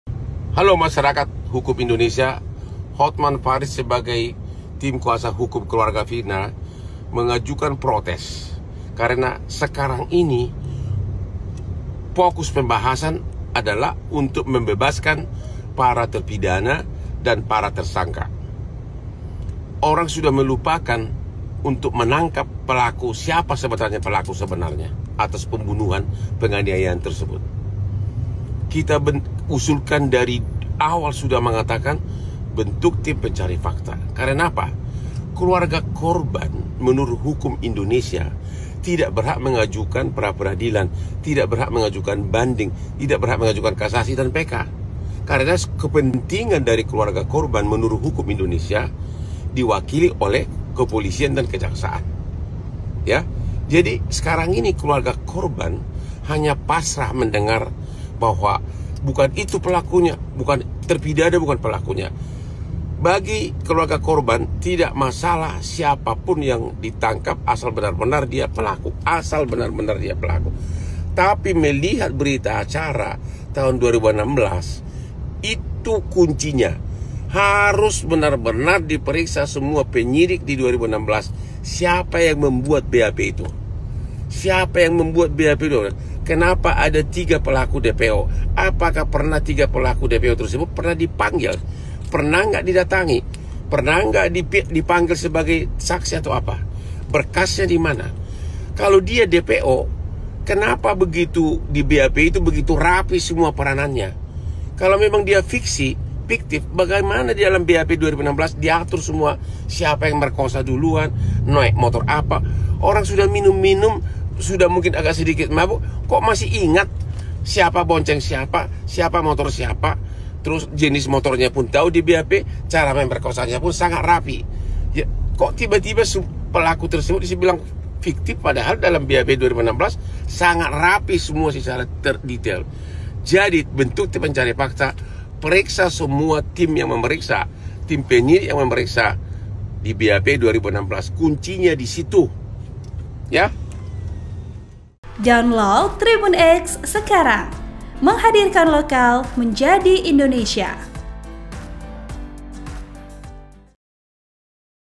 HOTMAN PARIS MURKA, Minta Penyidik Lama Kasus Vina Cirebon Diperiksa, Singgung Soal BAP!
TRIBUN-MEDAN.COM - Pengacara keluarga Vina, Hotman Paris melayangkan protes.

Dia meminta agar seluruh penyidik kasus tewasnya Vina pada 2016 diperiksa.

Sebab menurutnya kasus Vina ini semakin runyam dan tak terarah.

Demikian disampaikan Hotman Paris melalui akun Instagram pribadinya pada Jumat (12/7).

Hotman mengatakan saat ini fokus utama adalah untuk membebaskan para terpidana kasus Vina.

Hal ini menyusul dibebaskannya Pegi Setiawan dalam sidang praperadilan di PN Bandung.

"Orang sudah melupakan untuk menangkap siapa pelaku sebenarnya atas pembunuhan dan penganiayaan," kata Hotman.

Sejak awal memegang kasus Ini, Hotman sudah mengusulkan untuk membentuk tim pencari fakta.

Ia lantas mengungkit hukum di Indonesia, di mana keluarga korban tidak berhak mengajukan praperadilan, banding hingga Peninjauan Kembali (PK).

Sebab, kepentingan keluarga korban berdasarkan hukum di Indonesia telah diwakili kepolisian dan kejaksaan.

Hotman mengatakan keluarga Vina hanya bisa pasrah saat mendengar Pegi bukan pelaku yang sebenarnya.

Pihaknya pun berharap aparat bisa menangkap pelaku yang sebenarnya.

Lebih lanjut Hotman berbicara Berita Acara Pemeriksaan (BAP) 2016.

Ia menilai kunci terbukanya kasus Vina adalah BAP.

Hotman pun meminta aparat yang menyelidiki kasus ini untuk diperiksa terkait bagaimana penanganan kasus Vina saat itu.

"Tiga pelaku DPO apakah pernah dipanggil, didatangi, pernah enggak dipanggil sebagai saksi, berkasnya sebagai apa," kata Hotman.

Editor Video : M Andimaz Kahfi